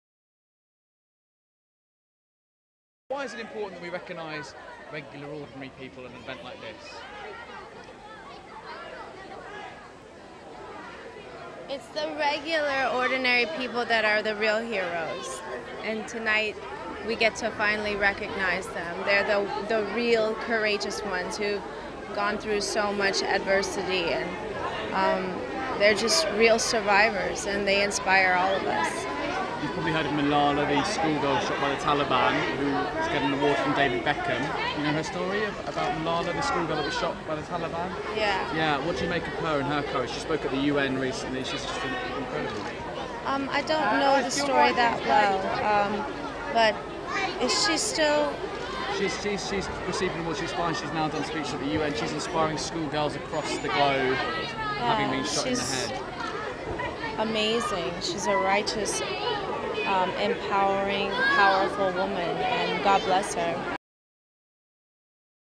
Malala I don't know that story well Nicole struggles with question
ITN reporter Darren Burn quizzed the 35-year-old from Honolulu at the Pride of Britain awards about the 16-year-old who was shot in the head and neck by the Taliban